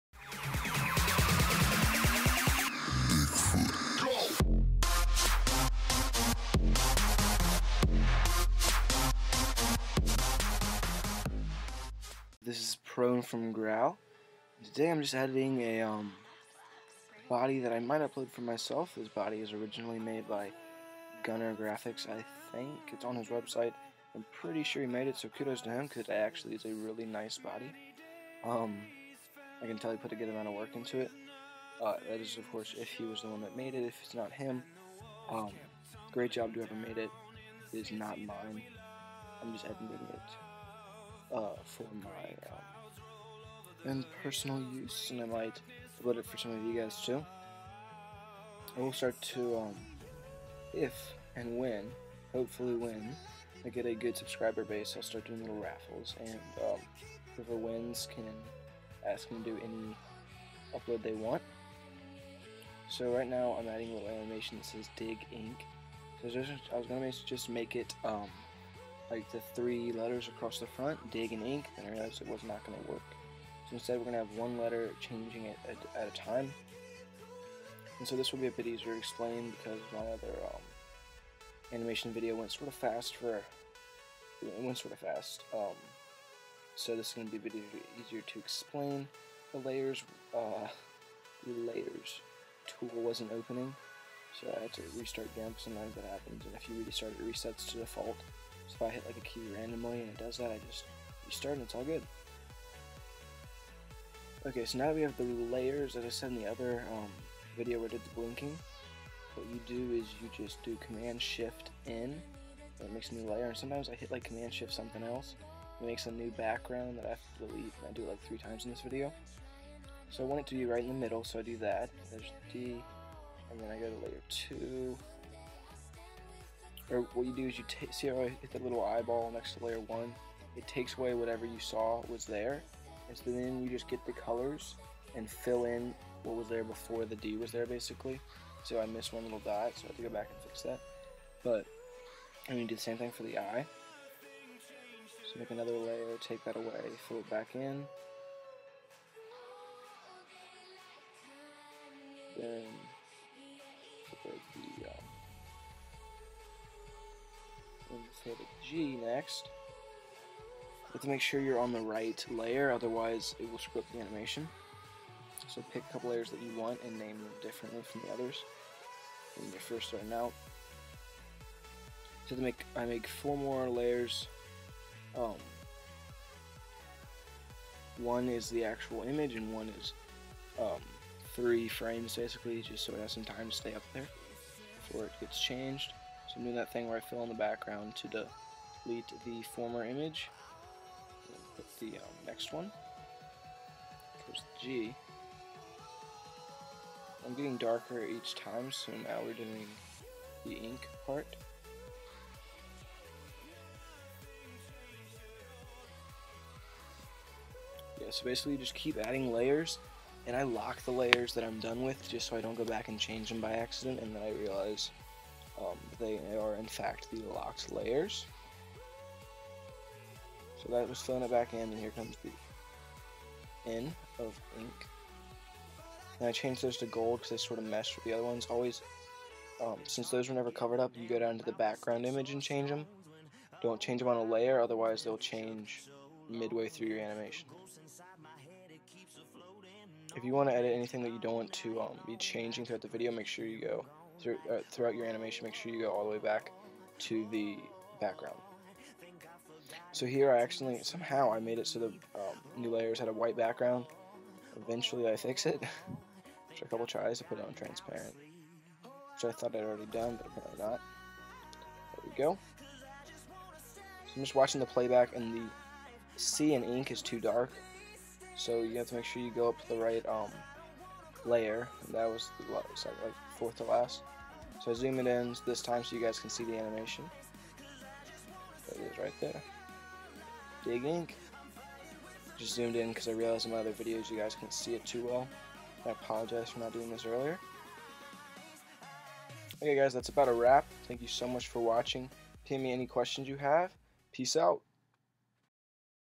Graal Animation Di
Graal body edit, along with a short animation tutorial. Enjoy